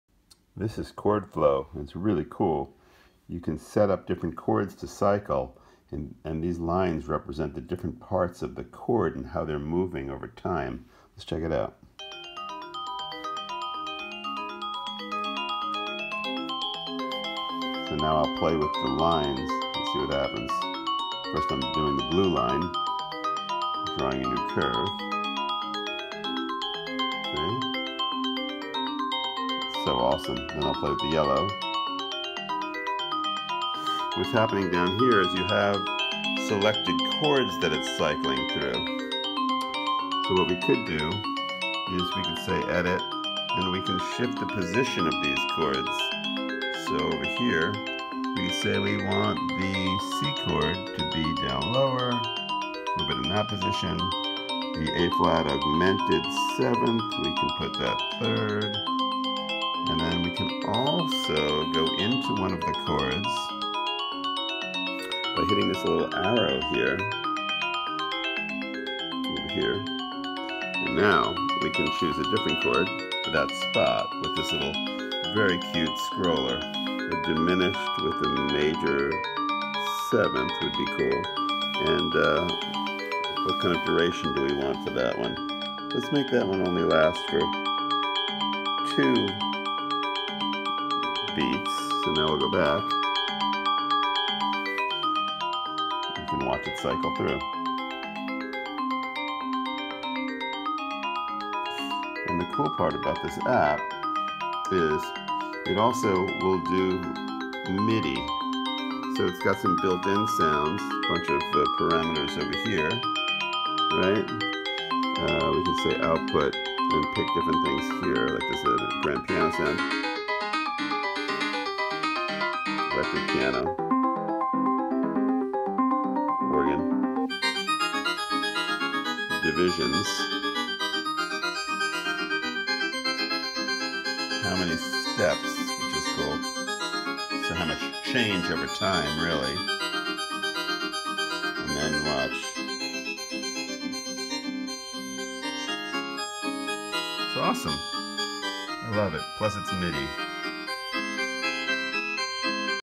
ChordFlow
ChordFlow is a chord sequencer with a unique 4-track polyphonic arpeggiator, extensive chord library, MIDI out and Ableton Link support. "It is intuitive, educational, inspirational and most of all FUN! From the moment I started the app up, it was a joy to explore." (Jordan Rudess)